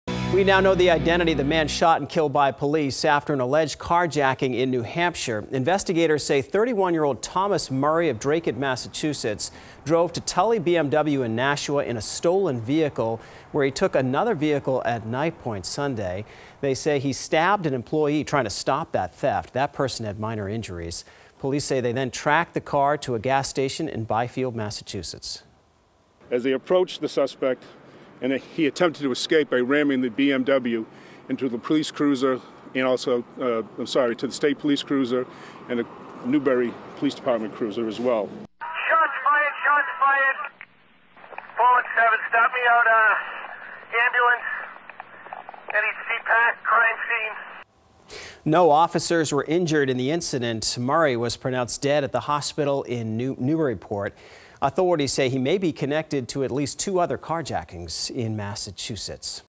Suspected carjacker killed by police identified
A man who was shot and killed in Massachusetts by police officers after an alleged carjacking in Nashua has been identified, officials said.